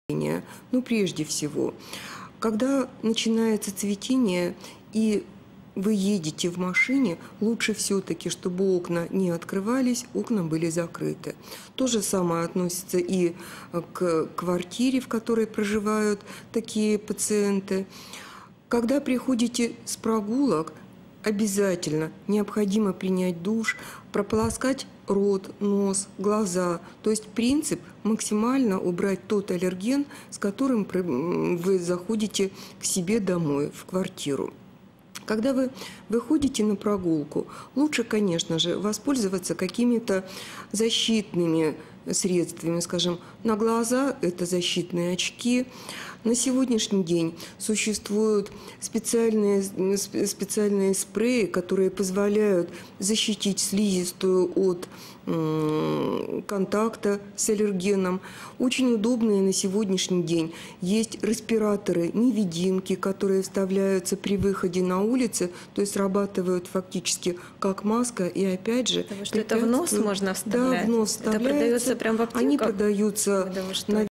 Как избежать аллергии на цветение без медикаментозного вмешательства?
ЦУ от аллерголога Липиной Валентины